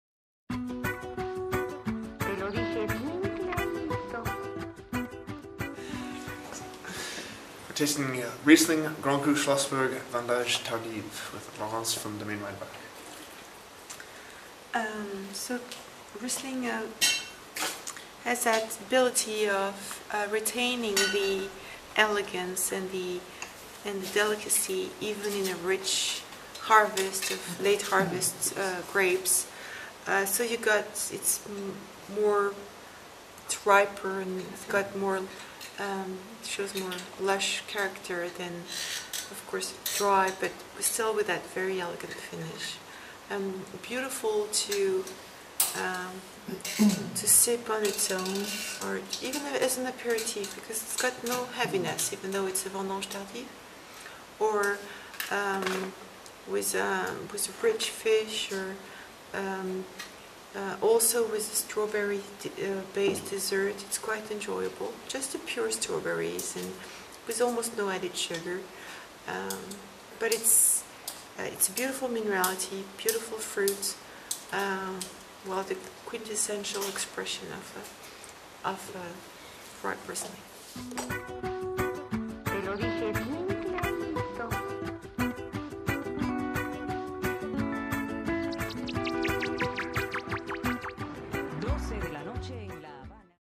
Domaine Weinbach Riesling GC Schlossberg Vendanges Tardives
Laurence Faller of Domaine Weinbach talks about their Riesling Grand Cru Schlossberg Vendanges Tardives.